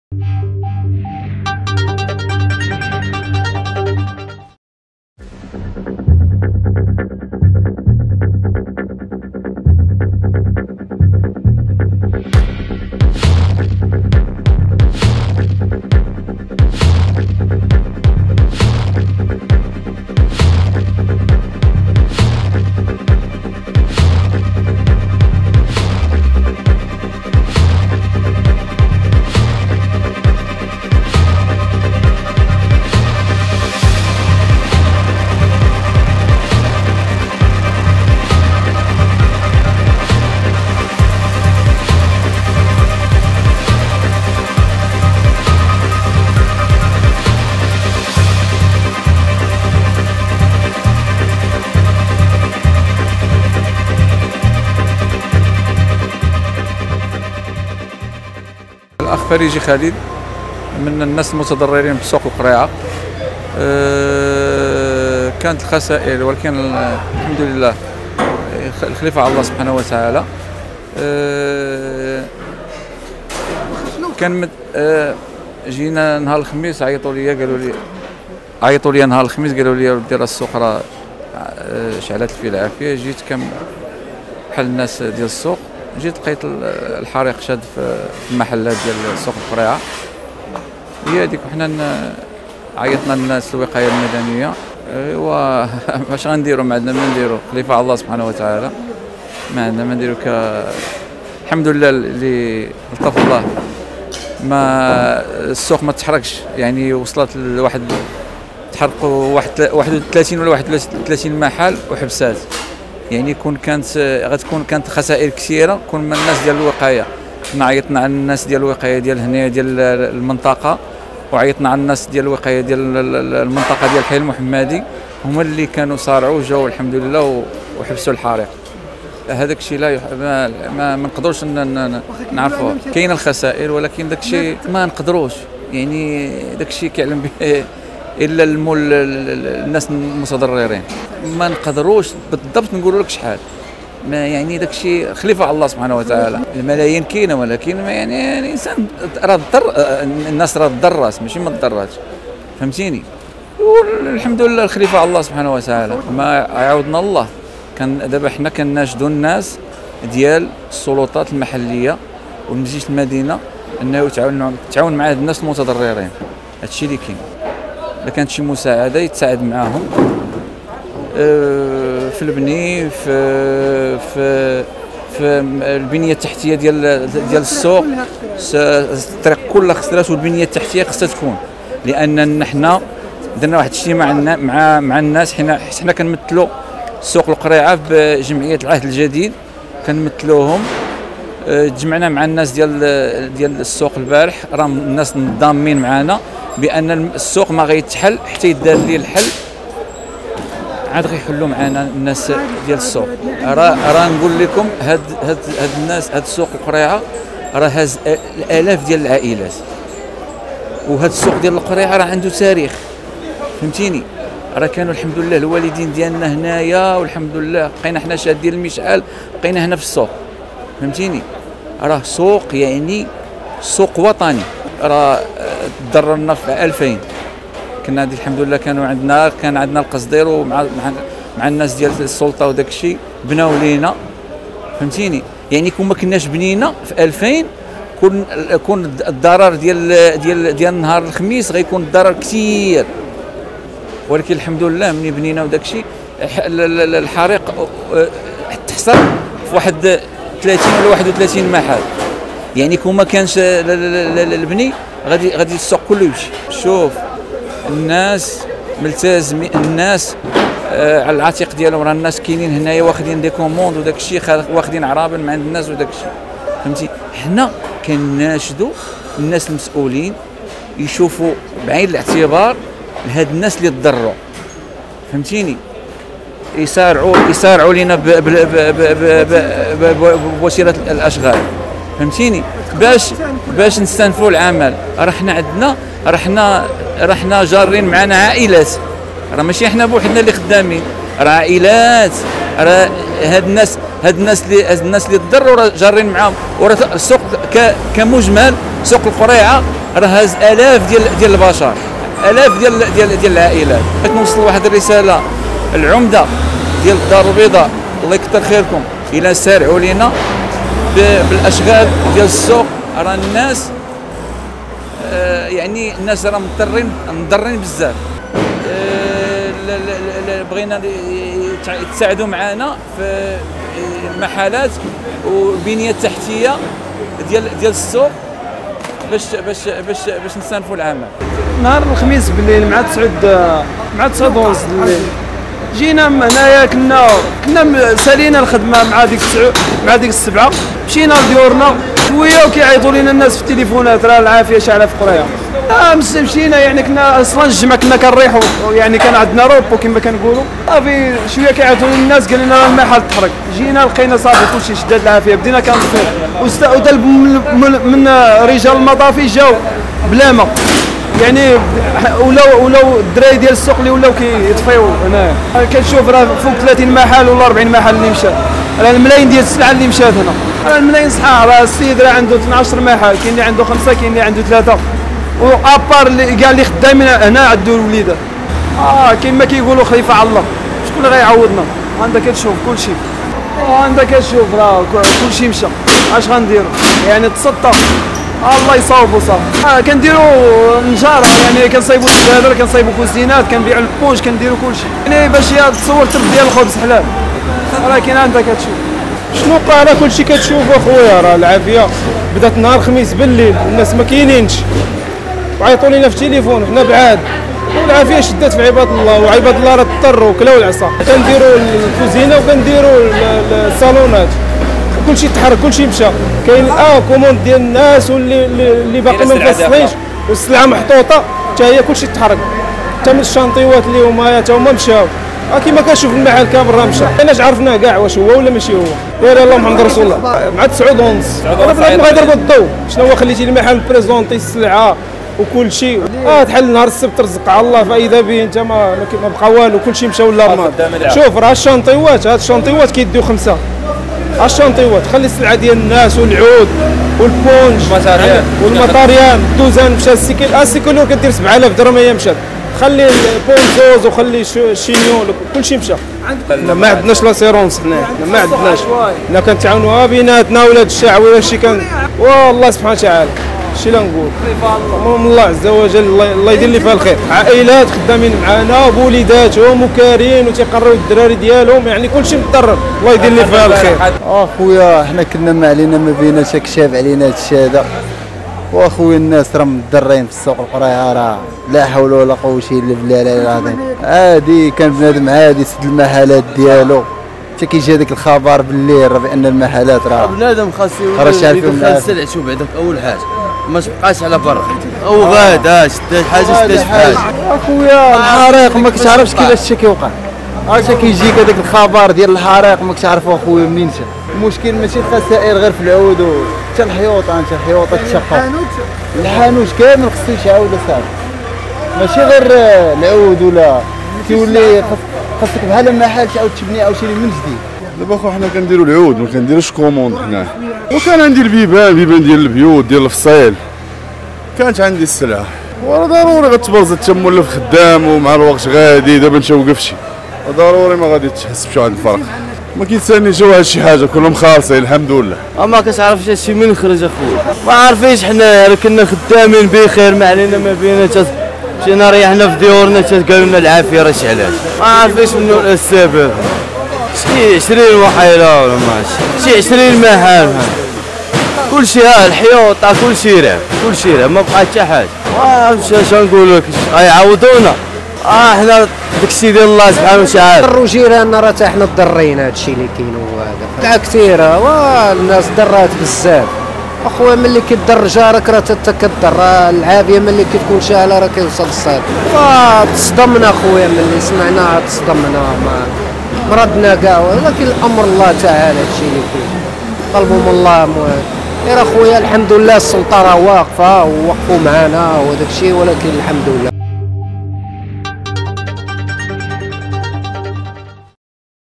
شوفو الخسائر الكبيرة والملايير لي مشات فسوق القريعة بعد الحريق المهول لي شعل بها والضحايا مصدومين
رصدت شوف تيفي الخسائر الفادحة التي وقعت بسوق القريعة بالدار البيضاء